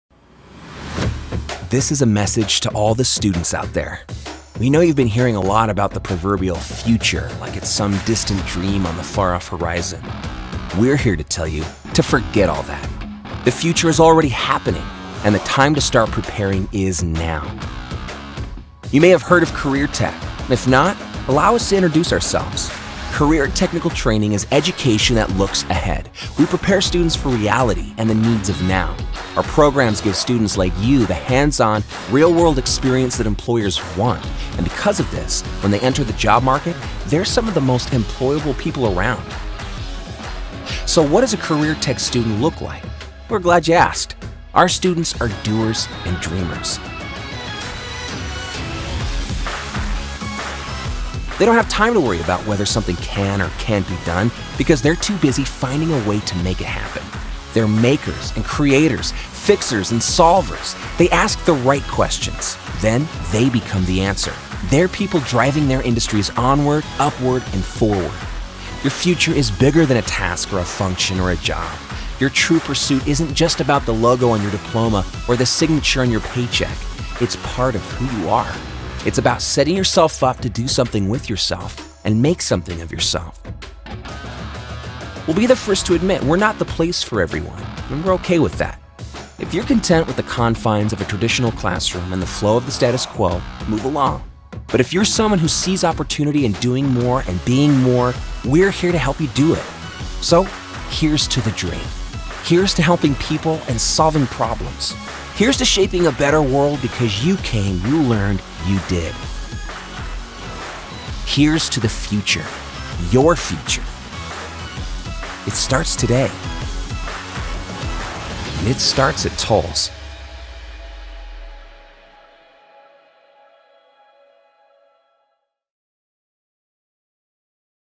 Find Your Pursuit at Tolles
So what does a career tech student look like? We’re glad you asked. Our students are doers and dreamers. They don’t have time to worry about whether something can’t or can’t be done, because they’re too busy finding a way to make it happen. They’re makers and creators, fixers and solvers. They ask the right questions – then they become the answer. They’re people driving their industries onward, upward and forward.

Your future is bigger than a task or a function or a job. Your true pursuit isn’t just about the logo on your diploma or the signature on your paycheck. It’s part of who you are. It’s about setting yourself up to do something with yourself and make something of yourself.